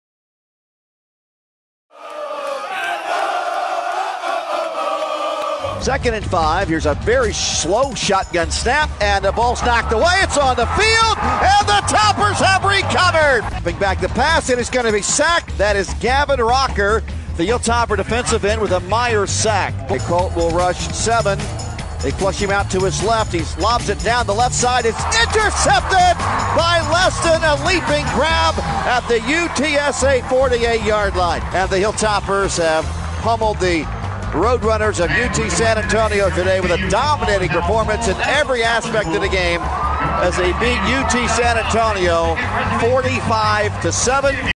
Hilltopper Defense Smothers UTSA on Senior Day
It was Senior Day on The Hill and the Hilltopper defense wasn't going to let the moment slip by them. WKU smothered the visiting Roadrunners from UTSA, holding them to a season-low seven points and 234 total yards. It was the largest margin of victory in an FBS conference game in school history and also made WKU bowl-eligible for the fourth consecutive season.

To follow the Hilltopper Football program, see us on Twitter, @WKUFootball.